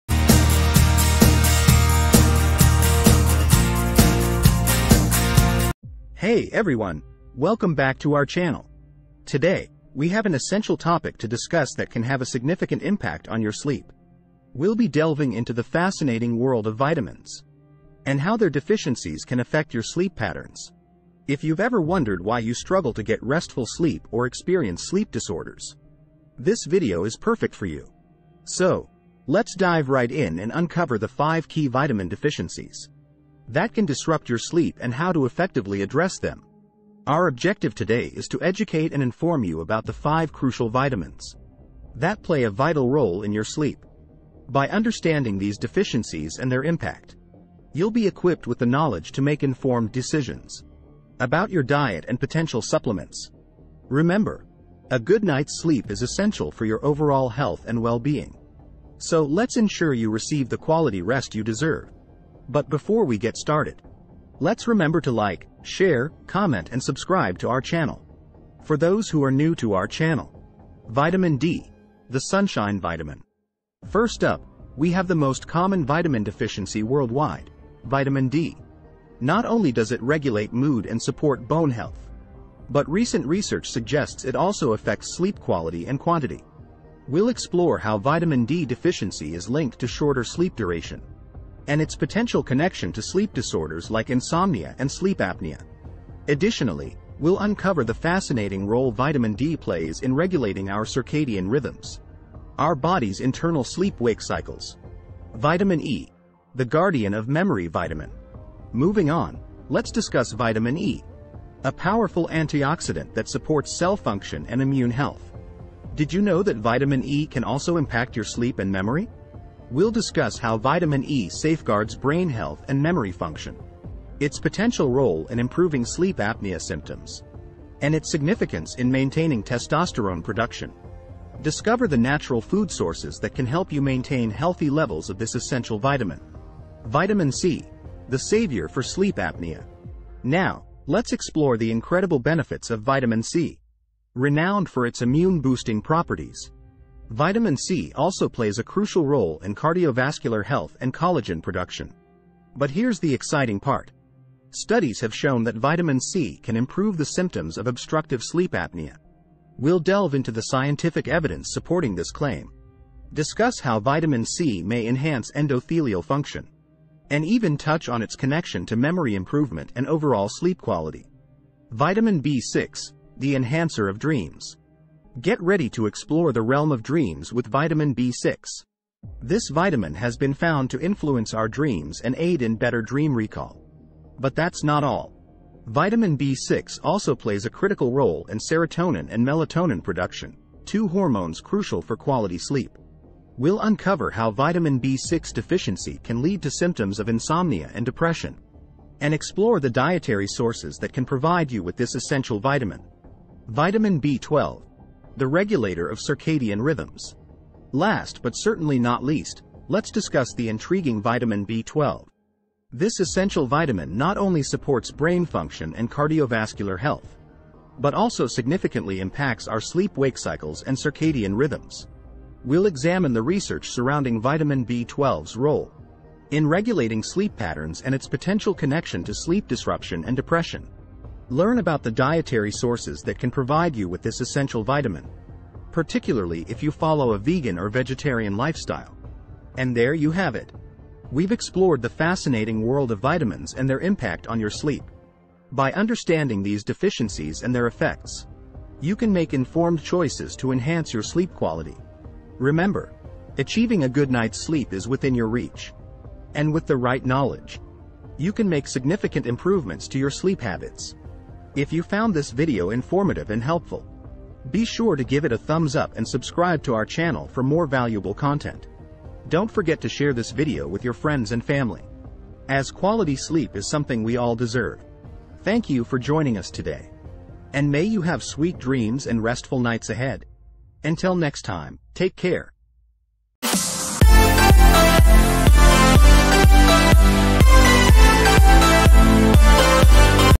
Sleep Better Tonight:The 5 Essential Vitamins for Restful Nights
This youtube video delves into the topic of vitamin deficiencies and their impact on sleep. Intending to educate and inform viewers, we discuss five crucial vitamins that play a vital role in sleep. The video covers vitamin D, vitamin E, vitamin C, vitamin B6, and vitamin B12.
The video starts by explaining how vitamin D deficiency, the most common deficiency worldwide, affects sleep quantity and quality. He explores its potential connection to sleep disorders like insomnia and sleep apnea, as well as its role in regulating circadian rhythms.
Moving on to vitamin E, an antioxidant highlights its impact on brain function, memory, and sleep apnea symptoms. He emphasizes the importance of maintaining healthy levels of vitamin E for testosterone production.
The video then explores the benefits of vitamin C, known for its immune-boosting properties. it discusses how vitamin C improves obstructive sleep apnea symptoms, enhances cardiovascular health, and may improve endothelial function. He also touches upon its connection to memory improvement and overall sleep quality.
Next, it explores the influence of vitamin B6 on dreams, dream recall, serotonin, melatonin production, and its role in insomnia and depression. He highlights the dietary sources of vitamin B6 that can help maintain healthy levels.
Finally, the video addresses the significance of vitamin B12 in regulating sleep-wake cycles and circadian rhythms. it discusses its connection to sleep disruption, and depression, and provides insights into dietary sources, particularly for individuals following a vegan or vegetarian lifestyle.
In the outro, the video reminds viewers to consult with their doctors or healthcare professionals before making any dietary or supplement changes. The ultimate goal is to empower viewers with the knowledge to enhance their sleep habits and achieve better sleep quality.